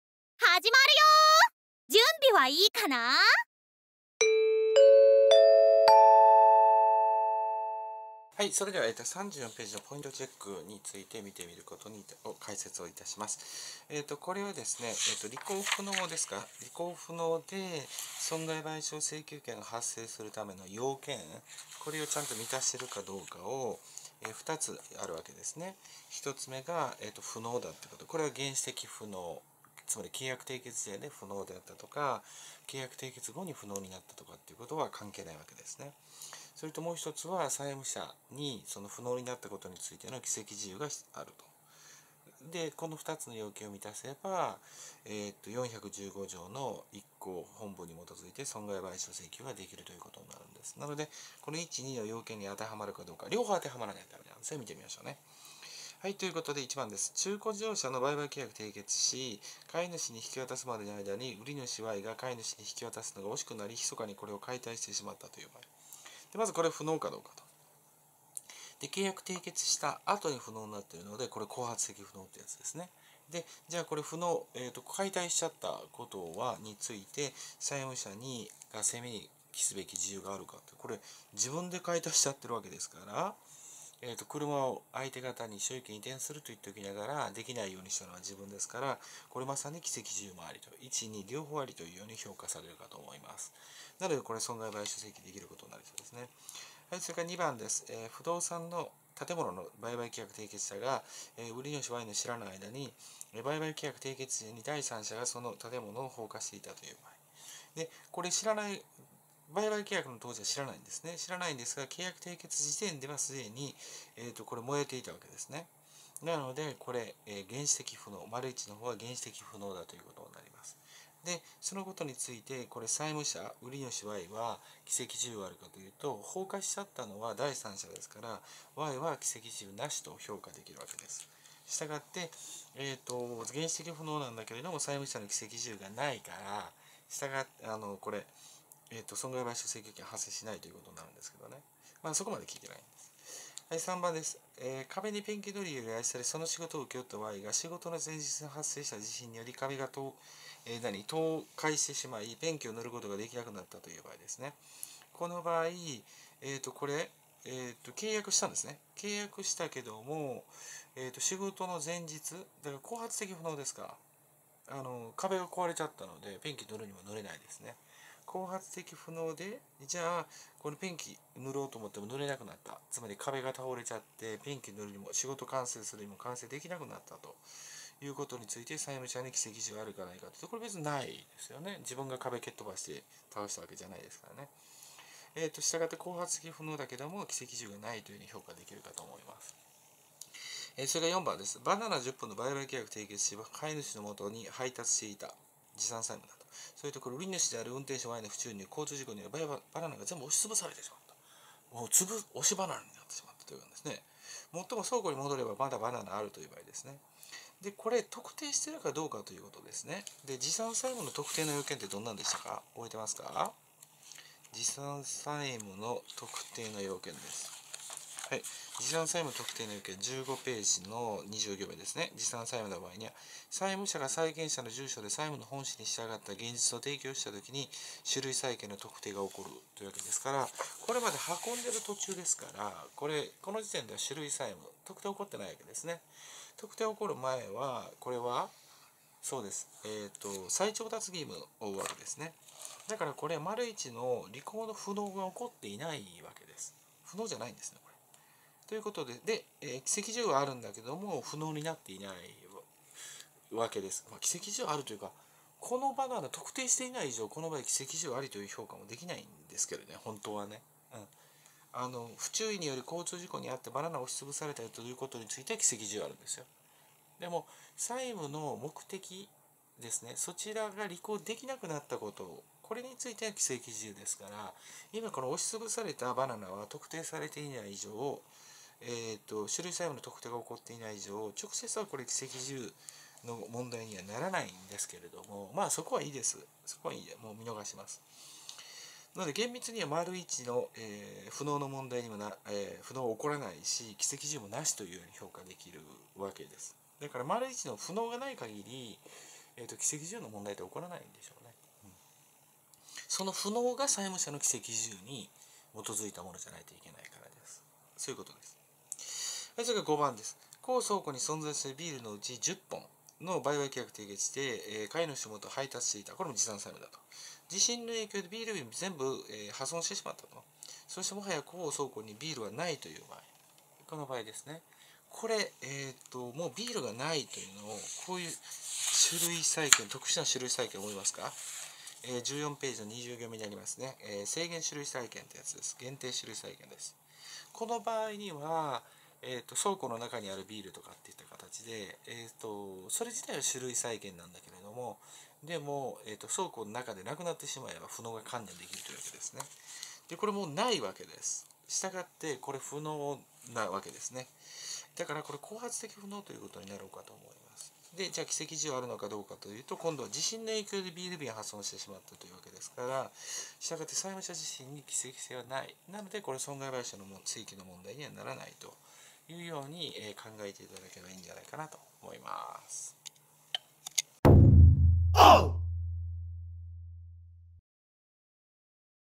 《問題解説編》パンデクテンシステムで学ぶ民法Ⅱ（債権） P34
「パンデクテンシステムで学ぶ民法Ⅱ（債権）〔第２版〕」の問題を解説しています。
ここではP34のPOINT　CHECKについて解説をしています。

テキストのお買い求めは、以下のamazonのHPよりどうぞ。